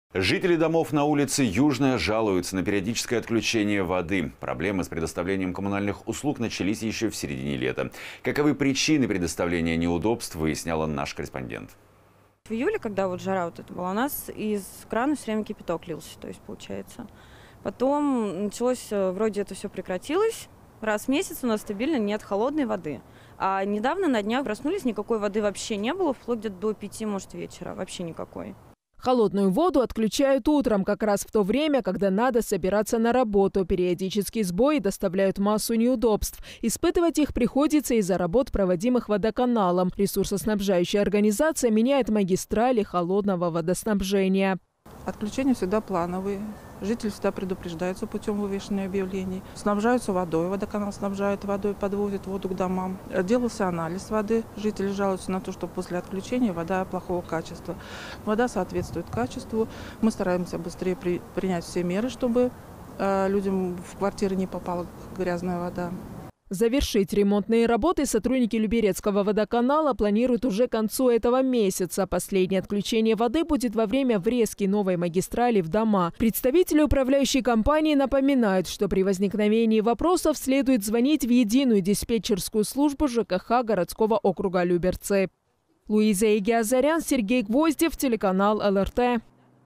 Без воды как без рук
Жители домов на улице Южная жалуются на периодические отключения воды. Проблемы с предоставлением коммунальных услуг начались еще в середине лета. Каковы причины предоставленных неудобств, выяснял наш корреспондент.

Анастасия Мурашова, жительница г.о.  Люберцы:
«В июле, когда жара вот эта была, у нас из крана все время кипяток лился. Потом вроде все это прекратилось. И раз в месяц стабильно у нас нет холодной воды. А недавно, на днях мы проснулись, никакой воды вообще не было вплоть до 5 вечера. Вообще никакой».

Холодную воду отключают утром, как раз в то время, когда надо собираться на работу. Периодические сбои доставляют массу неудобств. Испытывать их приходится из-за работ, проводимых водоканалом. Ресурсоснабжающая организация меняет магистрали холодного водоснабжения.

Татьяна Пыльнова, начальник ЖЭУ №5 АО «ЛГЖТ»:
«Отключения всегда плановые. Жителей всегда предупреждают путем вывешивания объявлений.  Водоканал снабжает водой, подвозит воду к домам. Делался анализ воды, жители жаловались, что после отключения вода плохого качества, вода соответствует качеству, мы стараемся быстрее принять меры, чтобы людям в квартиры не попала грязная вода».

Завершить ремонтные работы сотрудники люберецкого водоканала планируют уже к концу этого месяца. Последнее отключение воды будет во время врезки новой магистрали в дома. Представители управляющей компании напоминают, что при возникновении вопросов следует звонить в Единую диспетчерскую службу ЖКХ городского округа Люберцы.